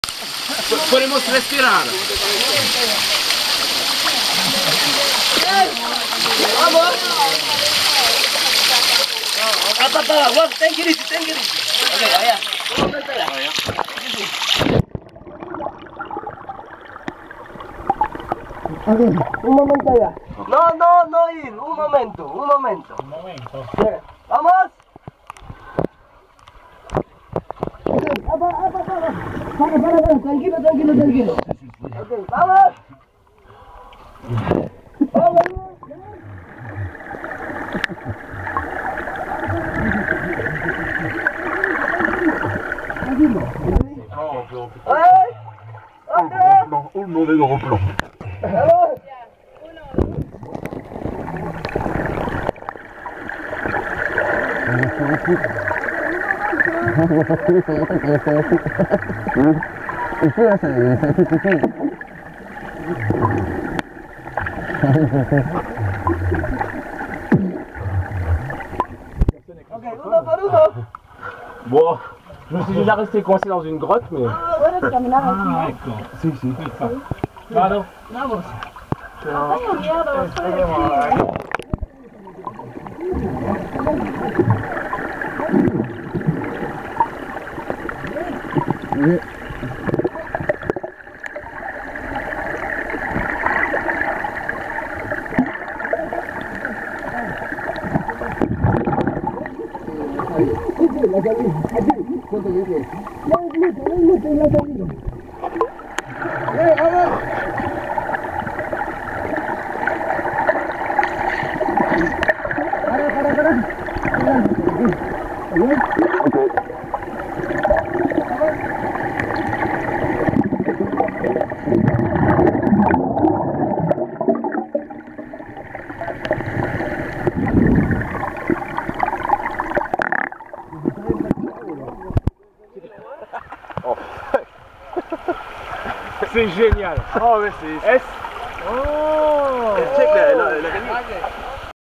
Under the cascade - Semuc Champey
diving under the cascade in semuc champey